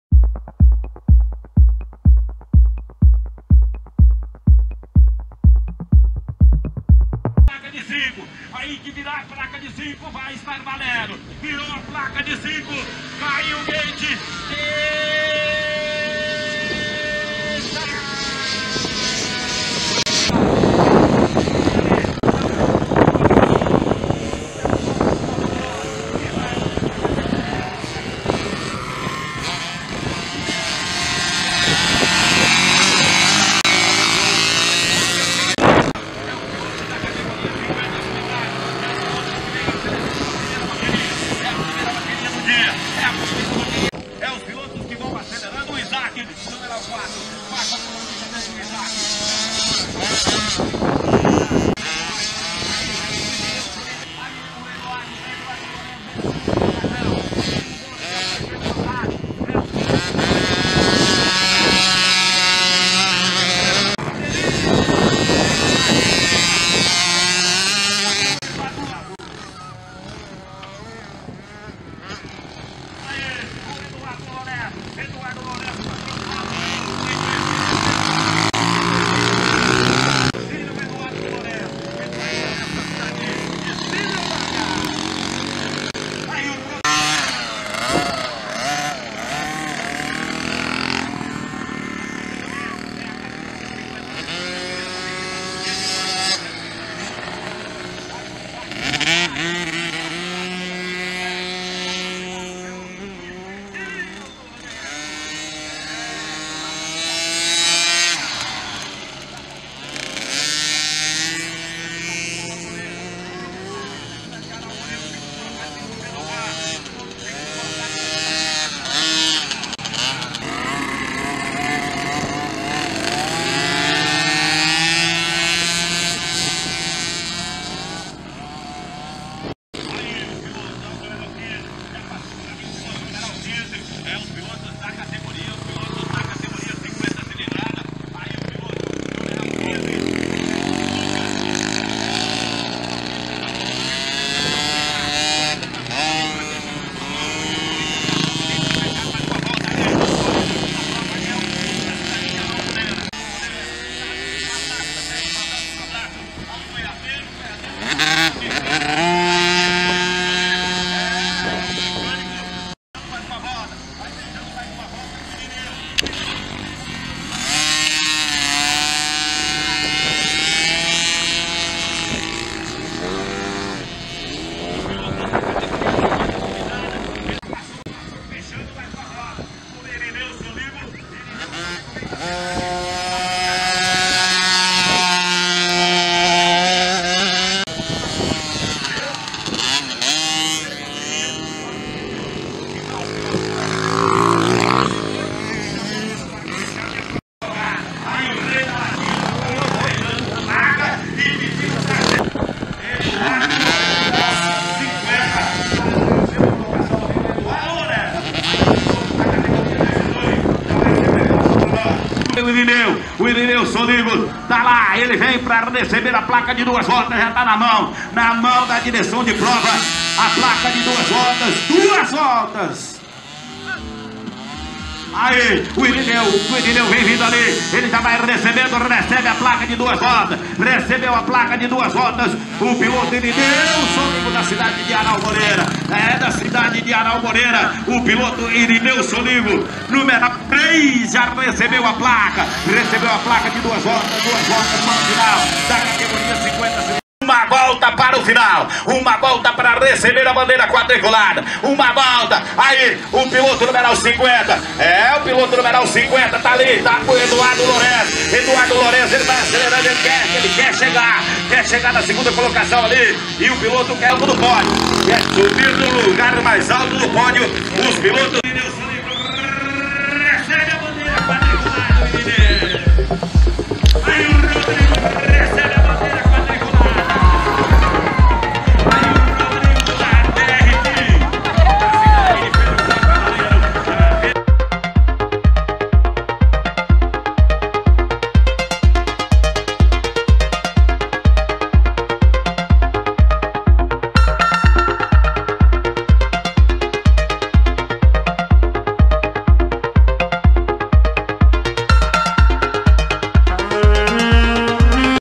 50cc em Naviraí pela 2º etapa da Copa Sulmems de Velocross 2024
Essa produção ocorreu graças aos parceiros do Blog Holeshot:

Ipê Dourado Restaurante
Universidad Columbia Medicina
Letícia MX
N.A Refrigeração